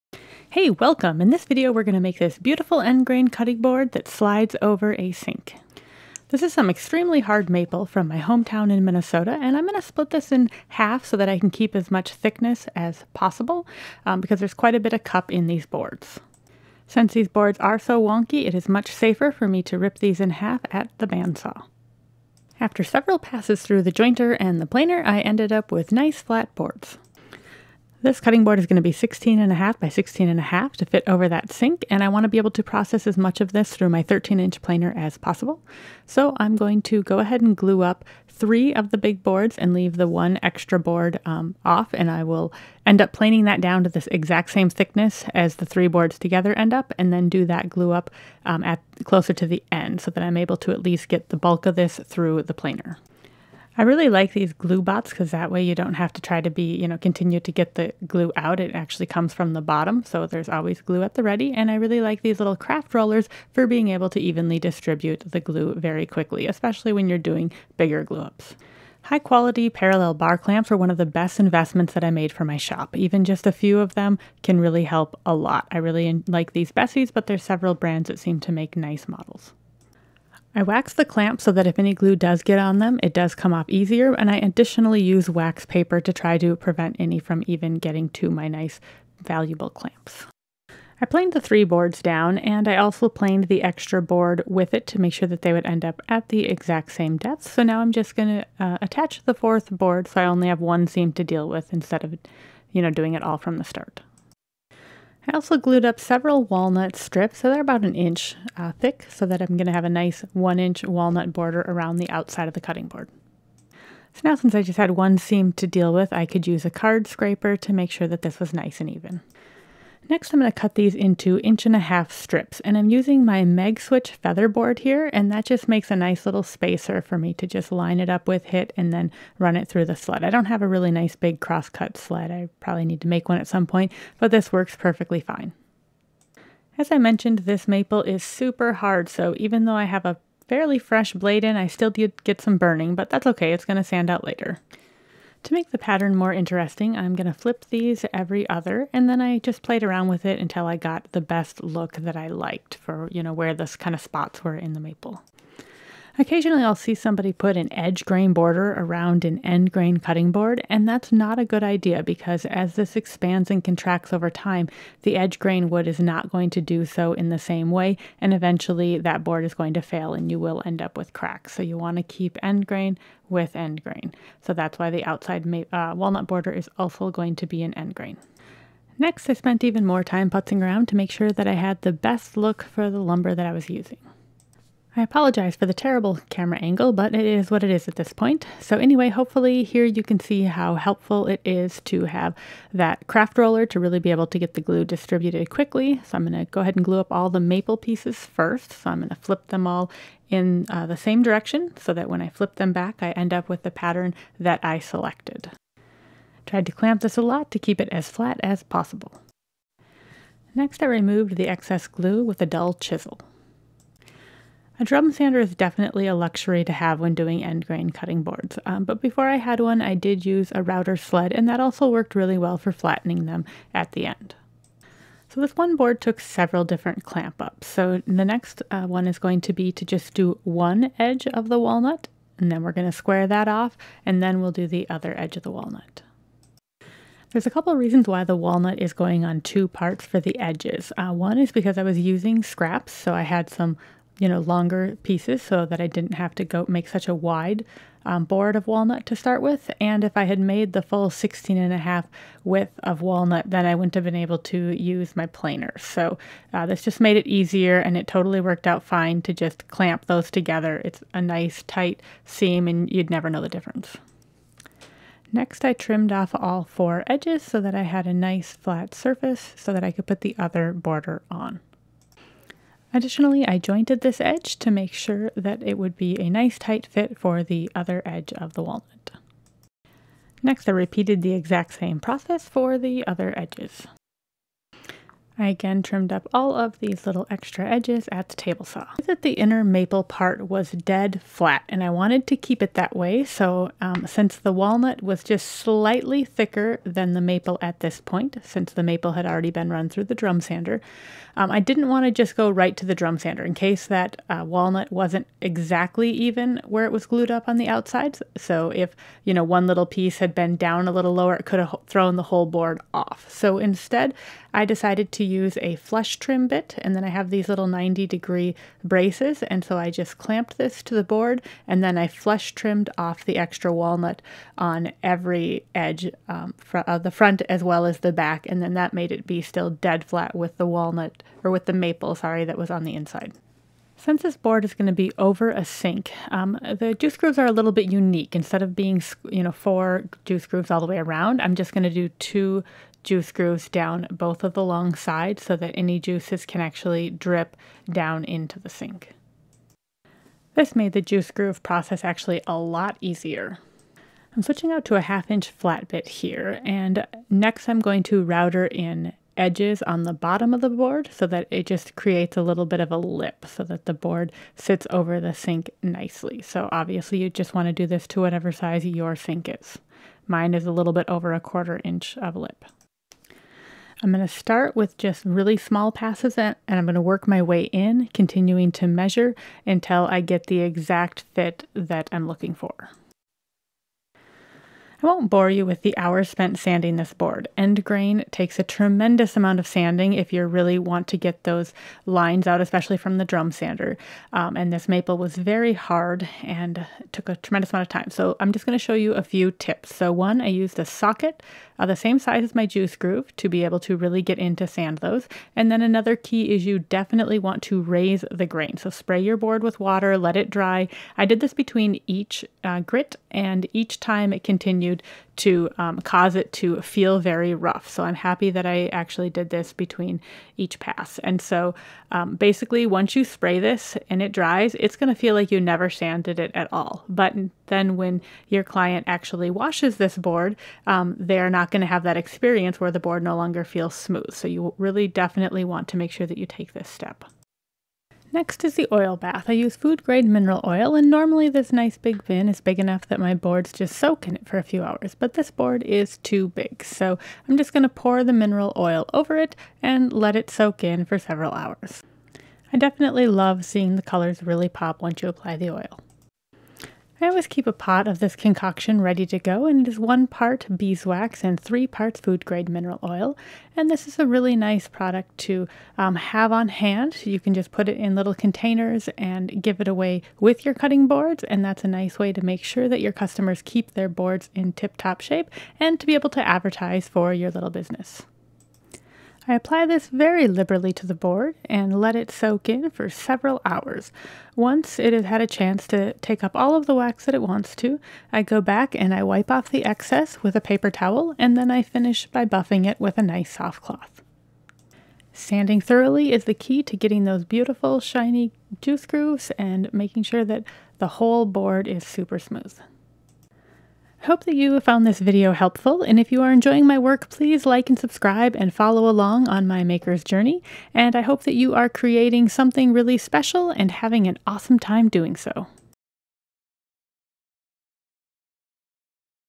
Over-The-Sink End Grain Cutting Board with Juice Grooves
This is a guide to building an end grain cutting board. This board is a unique design because it fits over a sink. Enjoy!

Products Used:

Bessey Clamps
Titebond II (Wood Glue)
Wood Glue (TiteBond II)
Food Grade Mineral Oil
Container for dipping boards and storage oil (air tight)
Craft Roller (for glue)